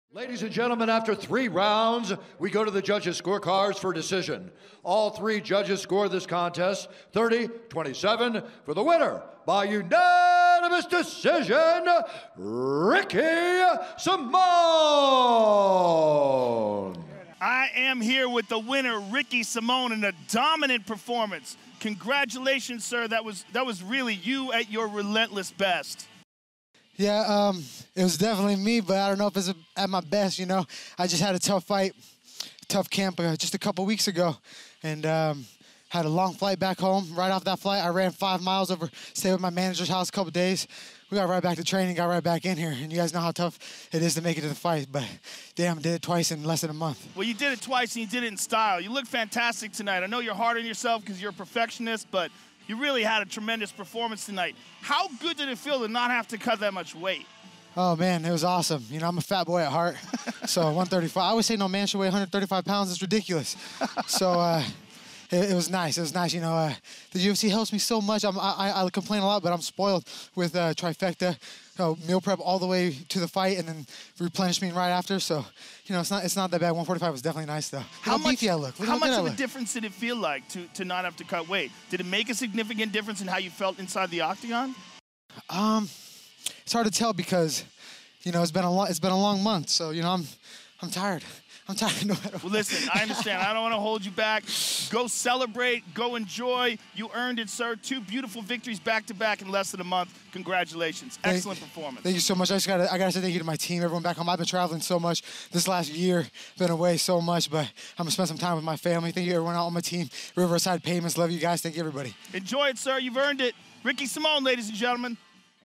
UFC 258: Ricky Simon - "145 Is Definitely Nice, Look How Good I Look"
Ricky Simon was in good spirits after his unanimous decision victory over Brian Kelleher at UFC 258. The victory was Simon's second win in less than a month.